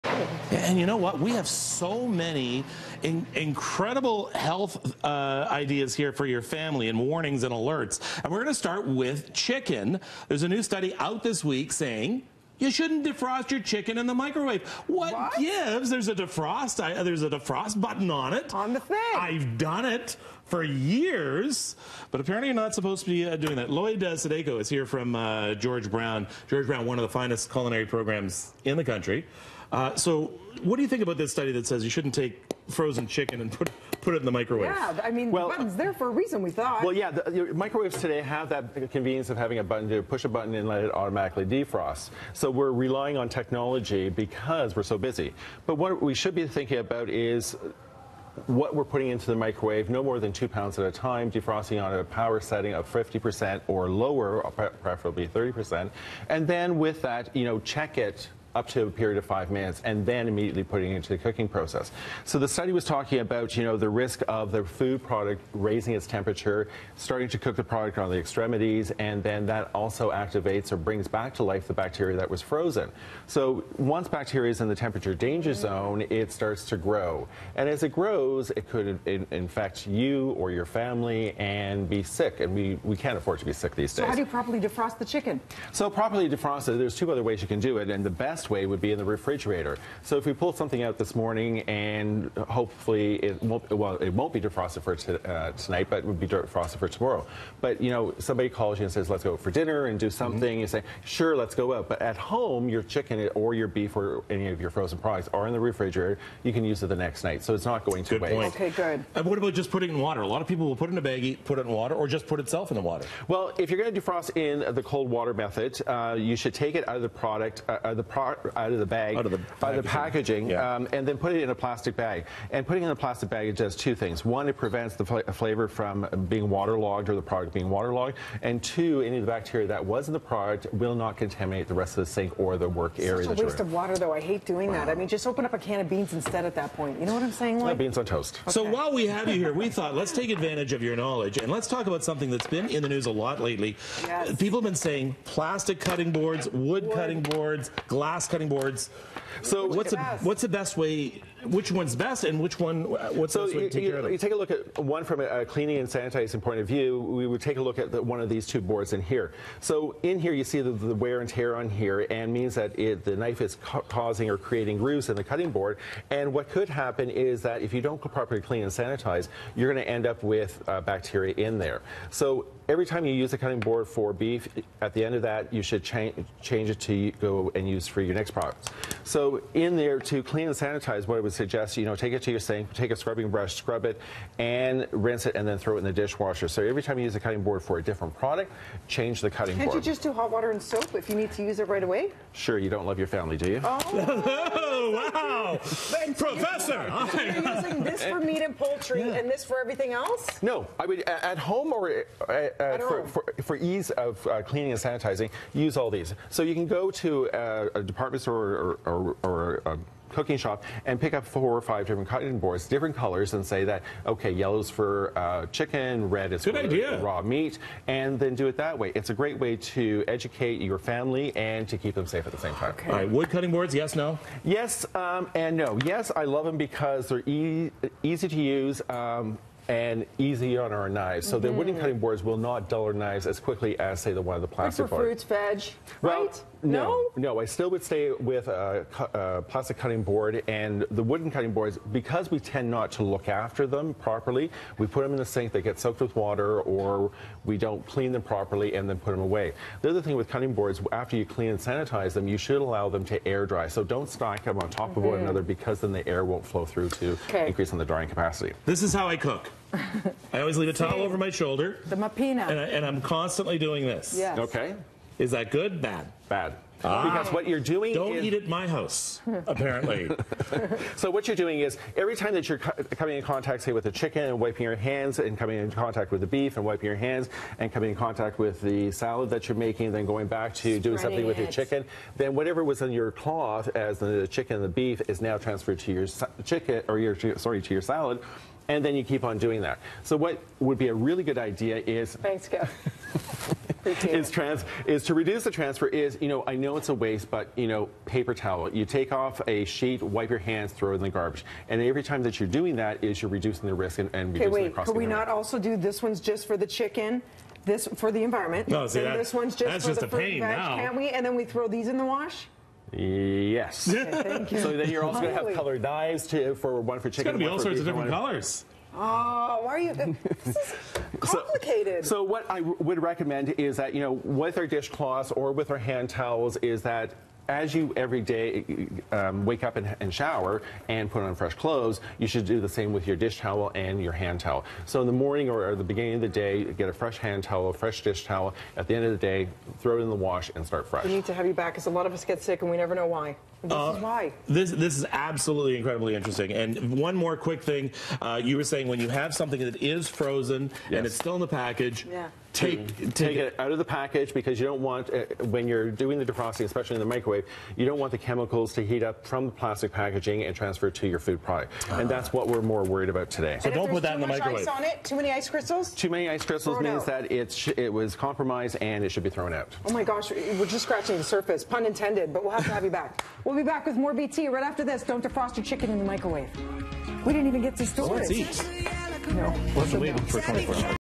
Food Safety 101 - How to handle and defrost meat
Lloyd Sudeyko, Professor/Program Coordinator, Food and Nutrition Management at George Brown College stops by to share how to safely handle and defrost food (hint: no chicken in the microwave!).